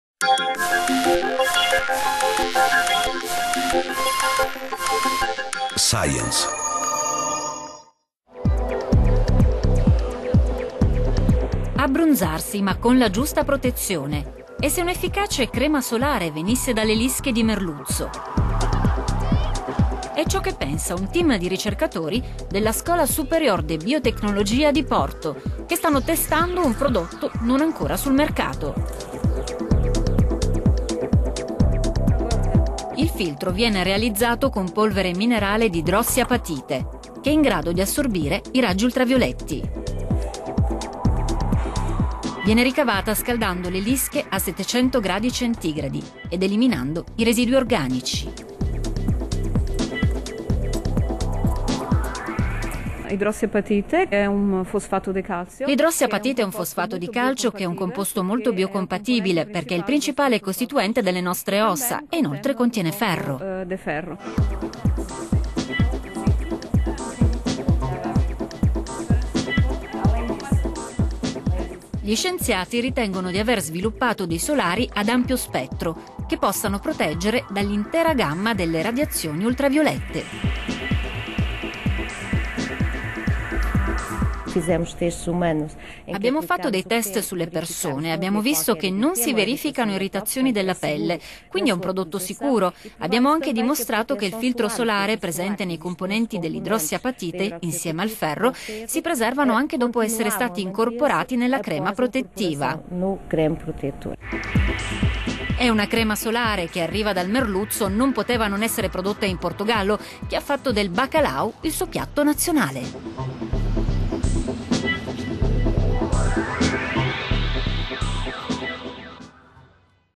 La crema solare che viene dal merluzzo - science
Abbronzarsi ma con la giusta protezione. E se un'efficace crema solare venisse dalle lische di merluzzo? È ciò che pensa un team di ricercatori dell'Escola Superior de Biotecnologia di Porto, che stanno testando un prodotto non ancora sul mercato.

1.26 SCHOOL OF BIOTECHNOLOGY, CATHOLIC UNIVERSITY OF OPORTO, PORTUGAL, LAB

Il filtro è prodotto con polvere minerale di idrossiapatite, che è in grado di assorbire i raggi ultravioletti.

Viene ricavata scaldando le lische a 700 gradi centigradi ed ...